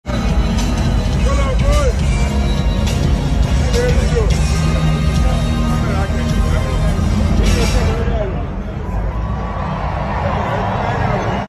Michael Jordan and Dennis Rodman SHARE SPECIAL MOMENT during NBA Top 75 Event!
Michael Jordan & LeBron James CELEBRATE with TOP 75 NBA Players of All Time!